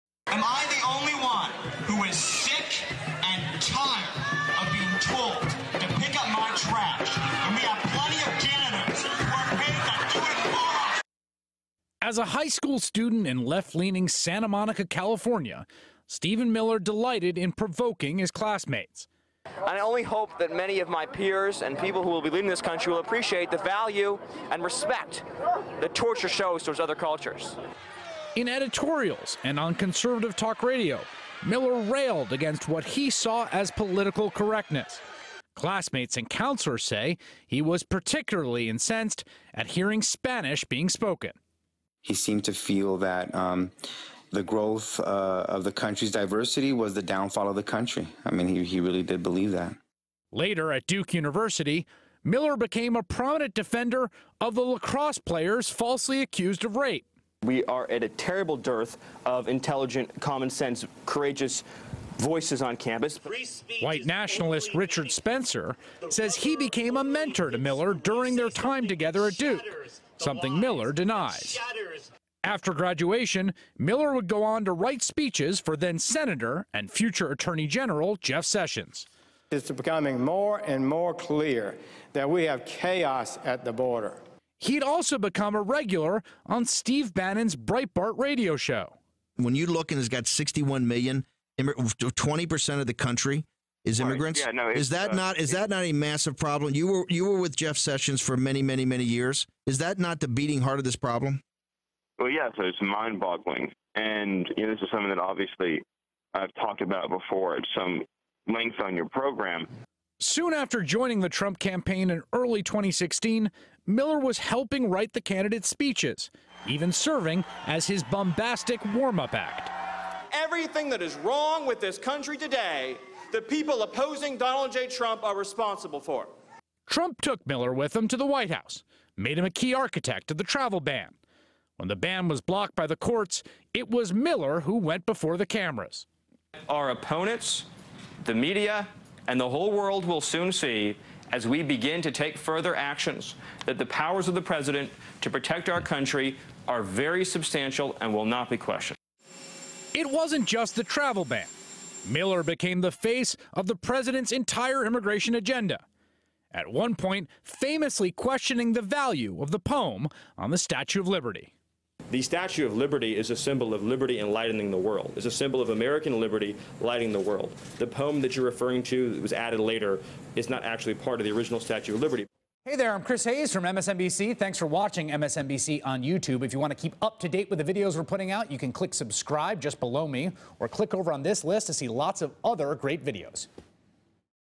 Meet Stephen Miller: The 32-Year-Old In The White House | MSNBC
How Stephen Miller went from trolling his classmates in high school...to President Trump's right hand man.